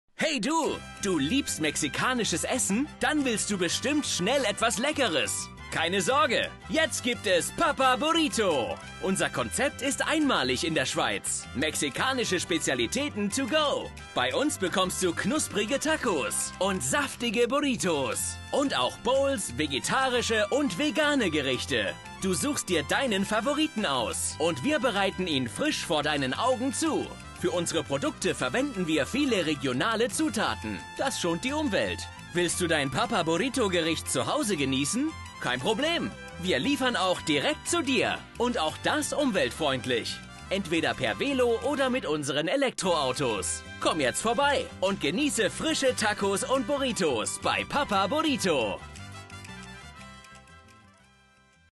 Papa Burrito in der Schweiz
Wir sind äusserst erfreut, dir unser neustes, in der Schweiz ein maliges Konzept präsentieren PAPA BURRITO.
Wir bieten dir typische mexikanische Spezialitäten TO GO. Bei uns bekommst du knusprige Tacos, saftige Burritos, traditionelle kubanische Sandwich, Bowls, vegetarische und vegane Gerichte.
Du suchst dir deinen Favoriten aus und wir bereiten Ihn frisch vor deinen Augen zu. Für unsere Produkte verwenden wir viele Regionale Zutaten, das schont die Umwelt. Willst du deine Papa Burrito Gericht zu Hause geniessen, kein Problem, wir liefern auch direkt zu dir und das auch umweltfreundlich.

📍 Kornhausplatz 14, Bern
🕙 Sonntag bis Donnerstag: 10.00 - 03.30 Uhr
🕙 Freitag und Samstag: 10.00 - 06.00 Uhr